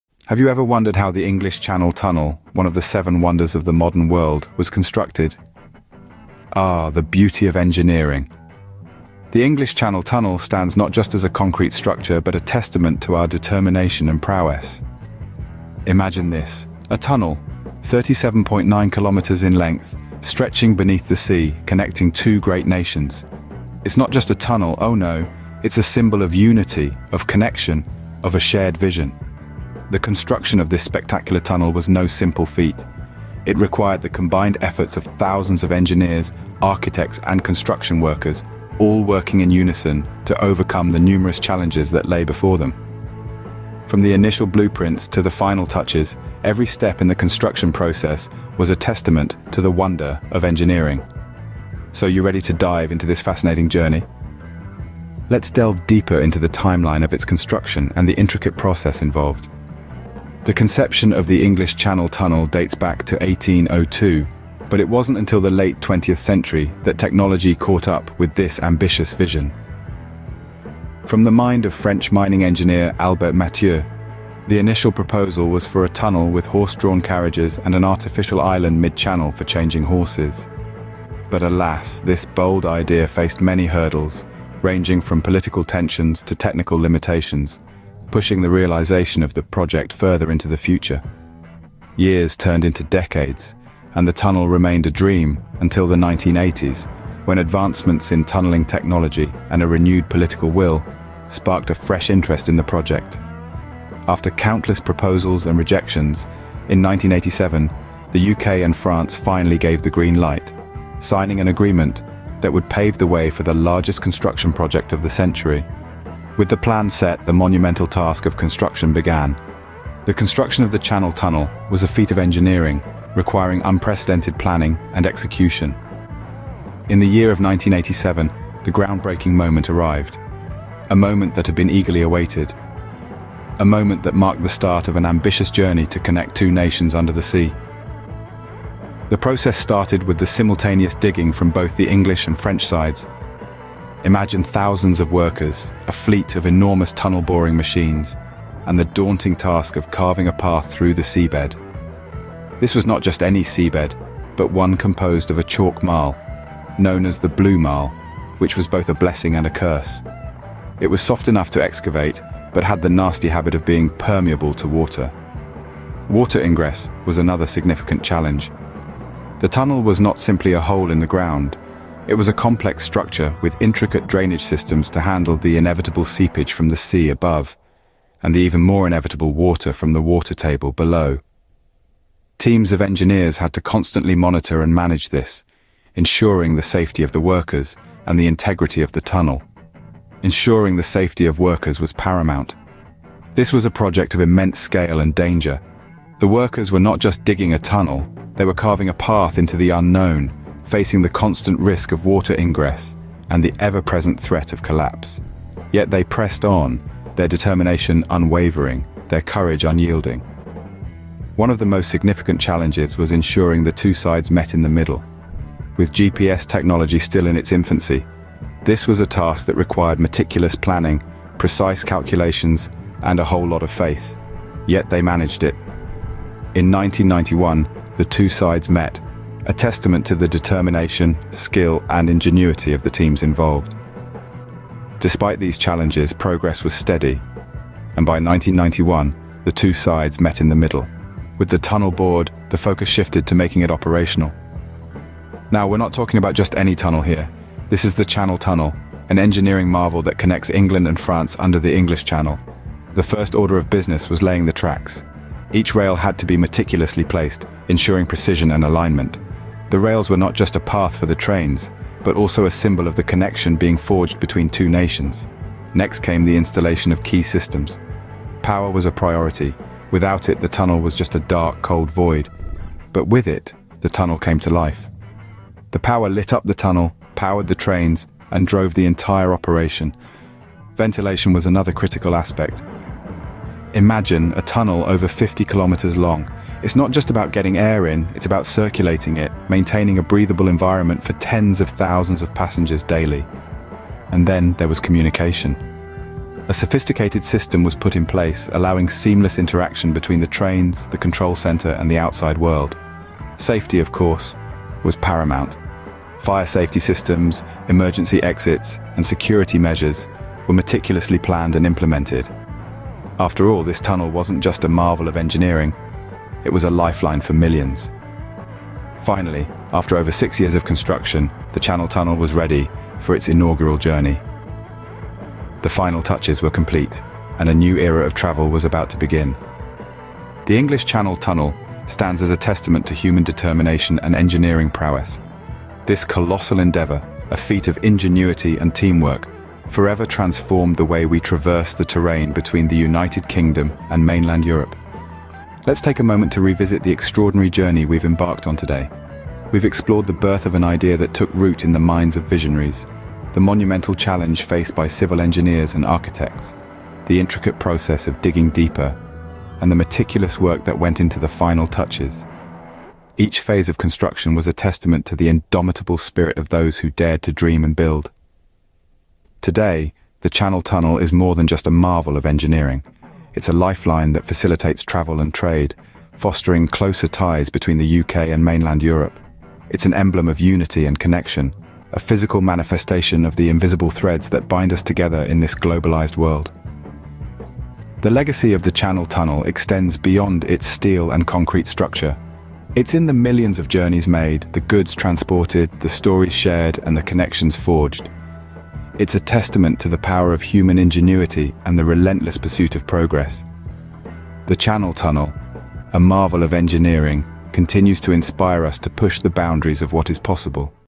Unbelievable Channel Tunnel Construction Secrets Revealed!
Prepare to be amazed as we unveil the incredible engineering and construction secrets behind the iconic Channel Tunnel. In this video, we delve deep into the remarkable journey of connecting two nations beneath the sea. Discover the ingenious design, cutting-edge technology, and the human ingenuity that made the Channel Tunnel a reality. Join us on this captivating exploration of one of the world's most remarkable infrastructure achievements